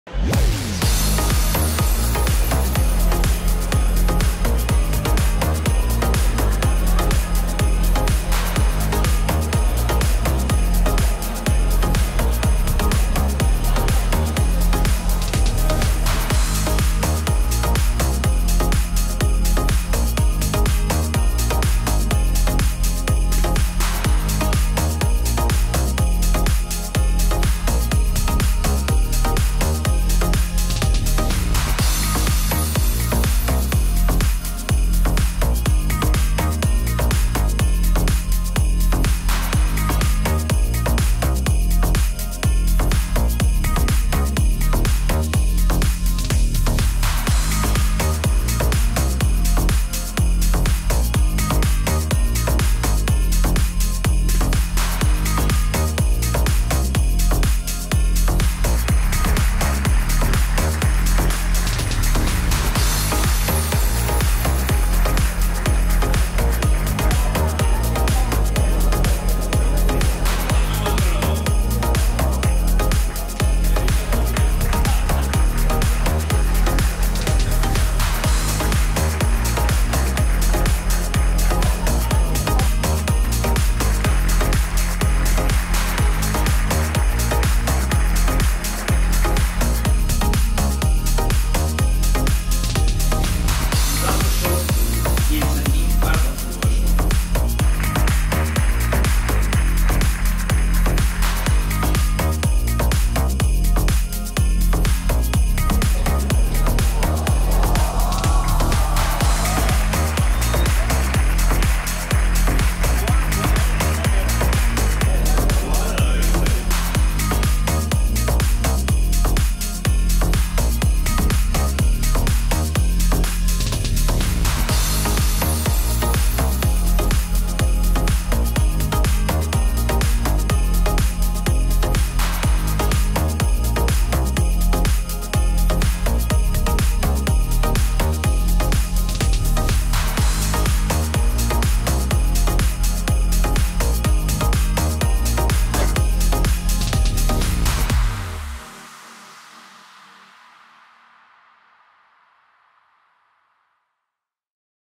Sopar Anual 2018
Resum del sopar que reuneix exjugadors del FC Barcelona.